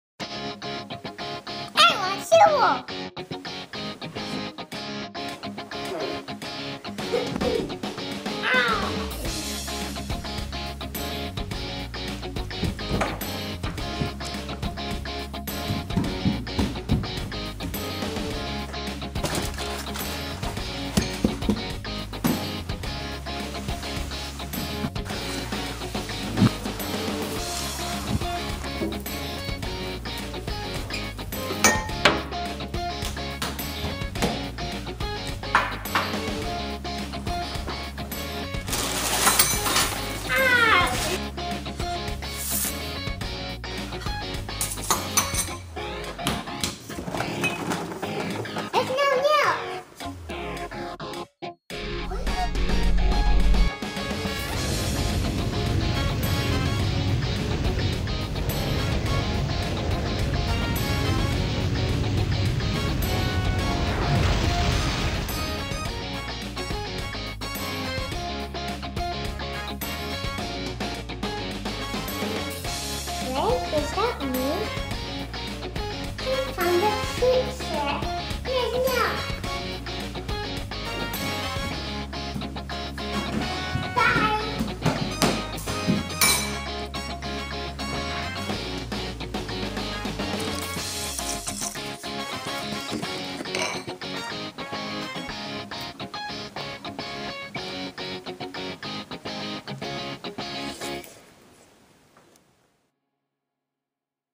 No Milk ~ Diode Short Film
What to do when you have a craving for cereal and there's no milk?
Introducing No Milk, starring my little sister Gielliesse.
Filmed in early 2013.